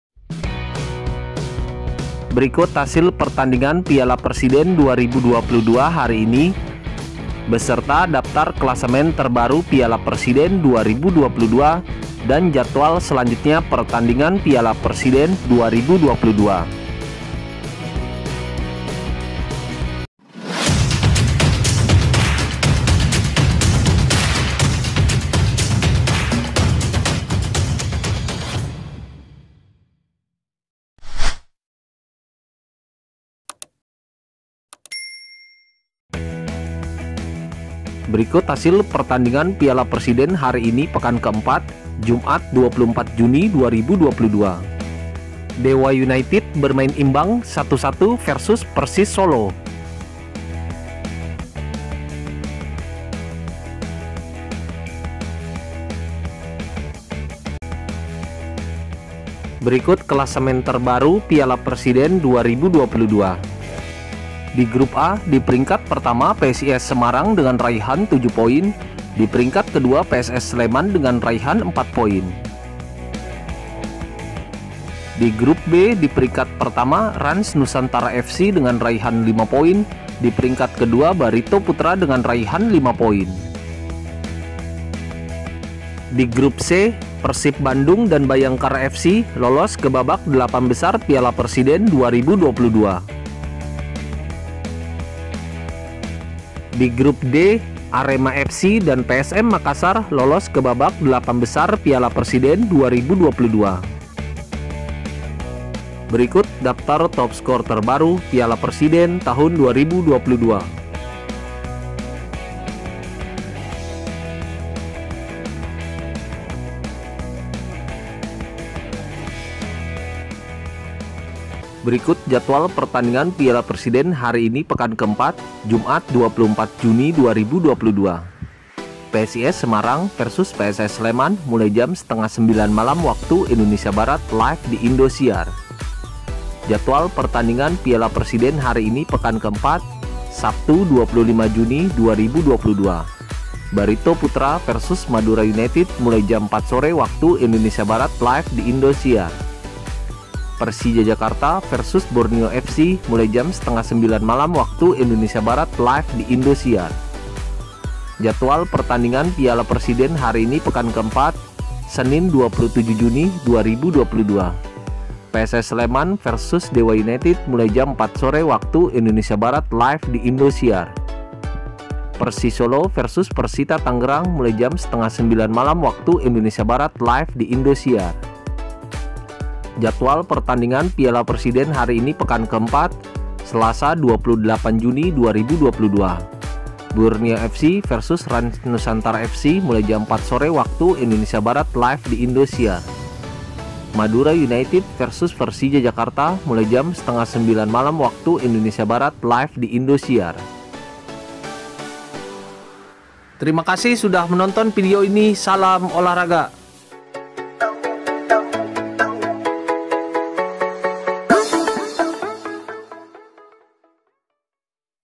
Hasil Piala Presiden Hari ini | Dewa united vs Persis | Jadwal Piala Presiden 2022 | Live Indosiar
Hasil Piala Presiden 2022 hari ini | Dewa united vs Persis | Klasemen Piala Presiden 2022 Terbaru | Jadwal Live Indosiar

Berikut hasil Piala Presiden 2022 hari ini pekan ke 4

Dewa United 1-1 Persis
Psis vs Pss

16.00 WIB: Barito vs Madura
20.30 WIB: Persija vs Borneo

16.00 WIB: Pss vs Dewa united
20.30 WIB: Persis vs Persita

16.00 WIB: Borneo vs Rans
20.30 WIB: Madura vs Persija

Semua pertandingan live indosiar
*jadwal sewaktu waktu dapat berubah